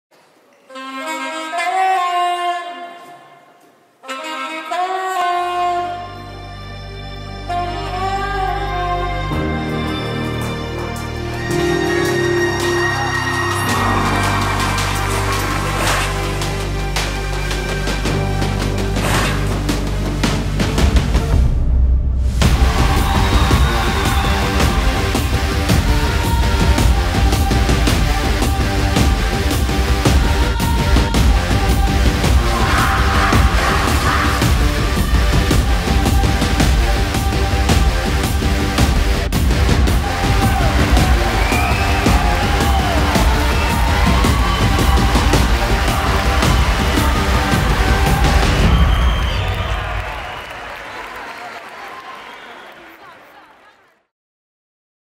The World Battle 2019 - Todos / Sonhos em Movimento (Day #2)
🕺Segundo dia da TheWorldBattle que decorreu no Coliseu do Porto com um Show de MomentumCrew e Gimnocerco (Todos - Sonhos em Movimento). Muito Obrigado à Câmara Municipal do Porto, Domus Social, Porto Lazer e ao Dr. Rui Moreira.